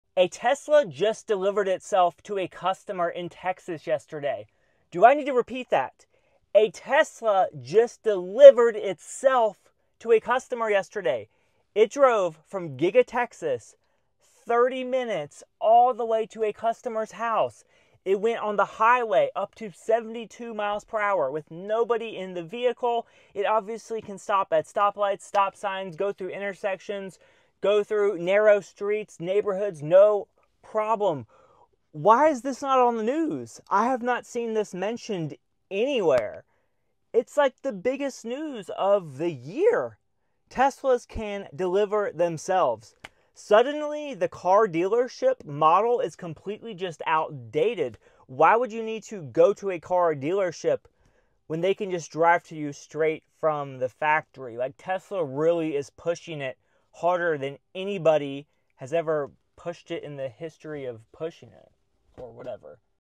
Tesla Robotaxi delivers itself to customer
A Tesla Model Y drove itself 30 minutes to a customer's house. It went from Giga Austin to their home. Why are more people not talking about this?